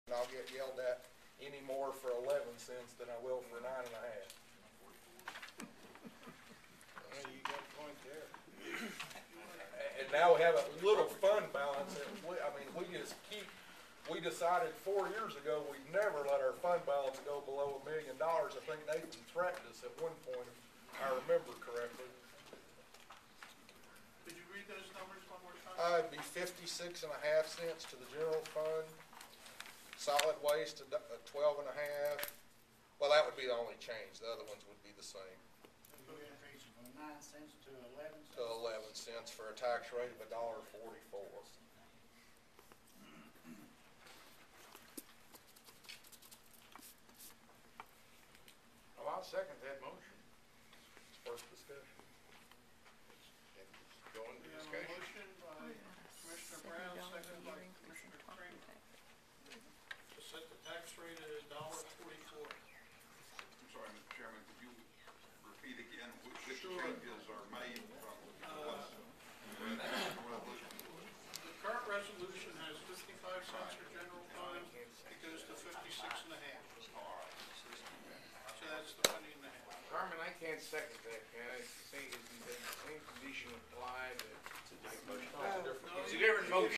Commissioner Jeff Brown proposes an 11 cent property tax increase
Cumberland County Commissioner Jeff Brown proposed to raise the property tax 11 cents instead of 9.5 cents saying he wouldn't get fussed at anymore for the additional tax increase.  Is proposal would have funded schools, landfill, library funding and would have increased the very small $700,000 cushion in the fund balance of the budget.